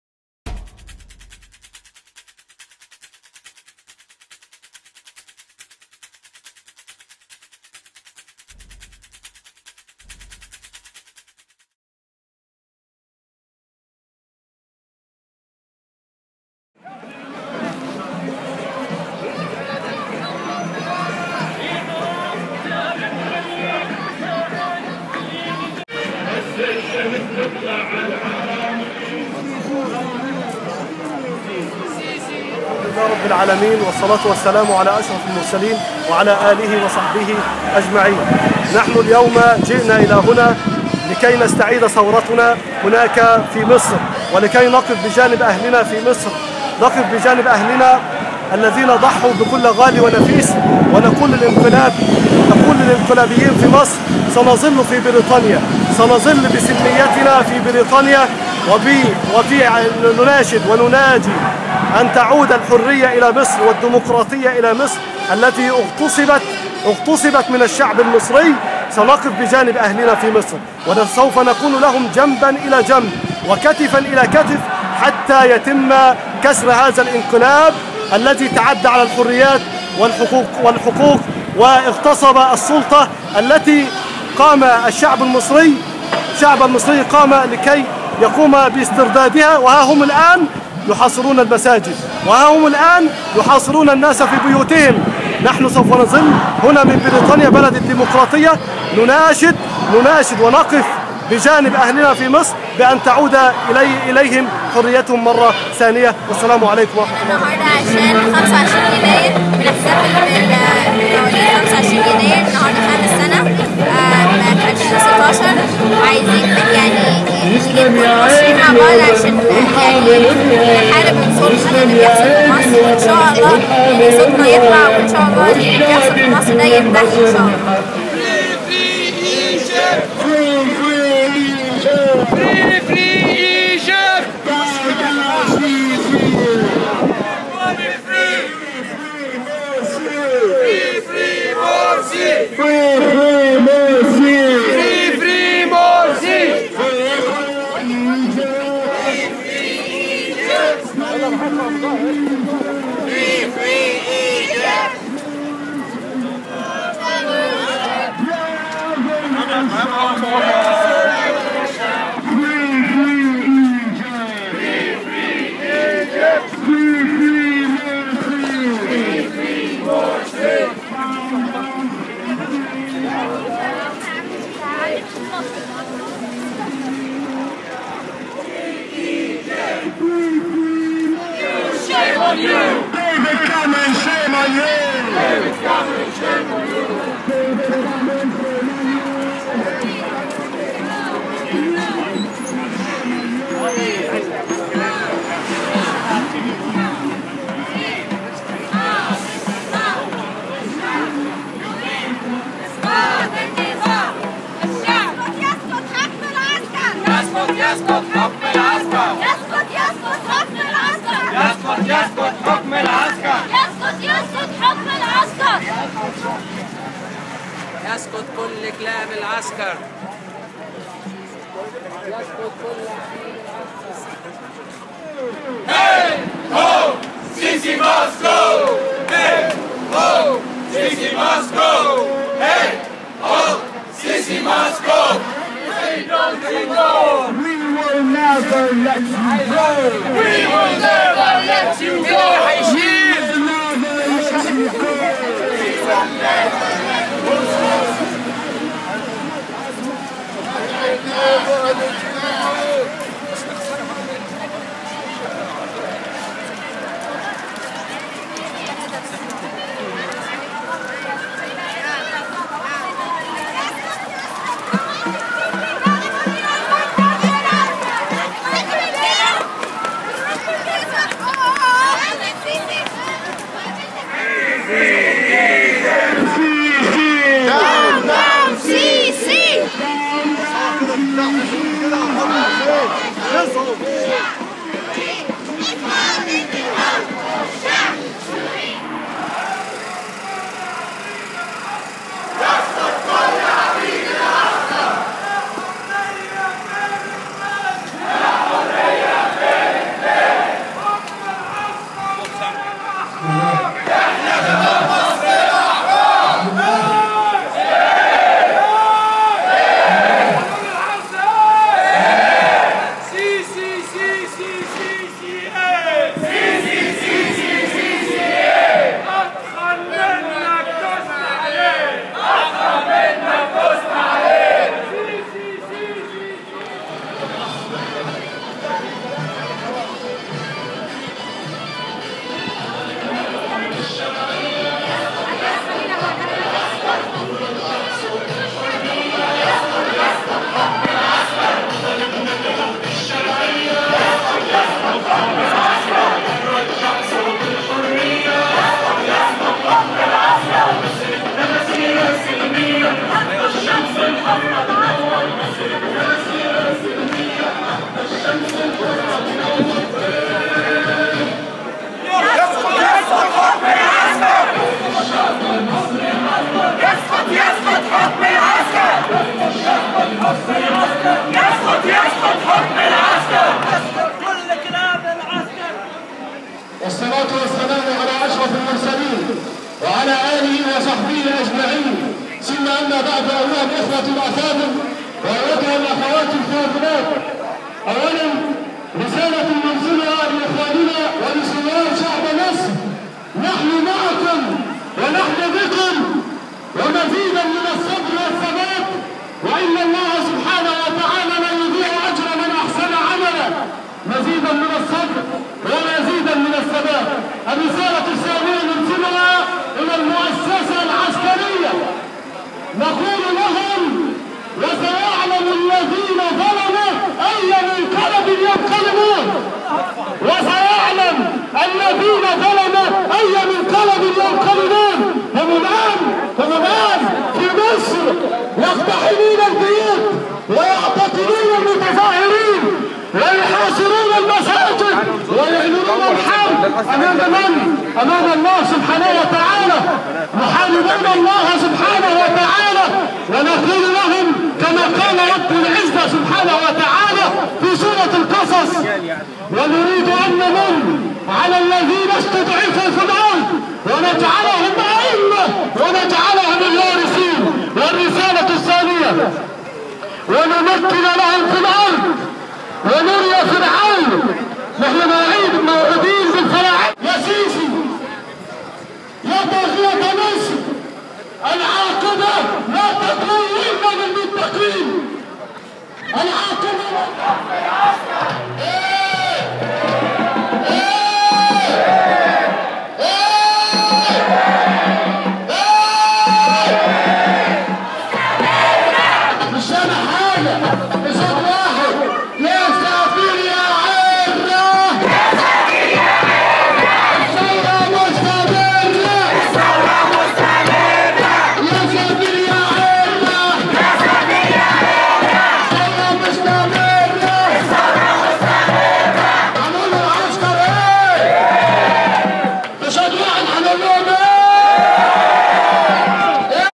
فيديو من مسيرة اليوم لإحياء الذكرى الخامسة لثورة يناير أمام سفارة مصر بلندن
سوف يكتب التاريخ. ان. وزير دفاع مصري. بمساعدة دول اجنبيه خان بلاده وخان رئيسه وشعبه وخان القسم الذي اقسم عليه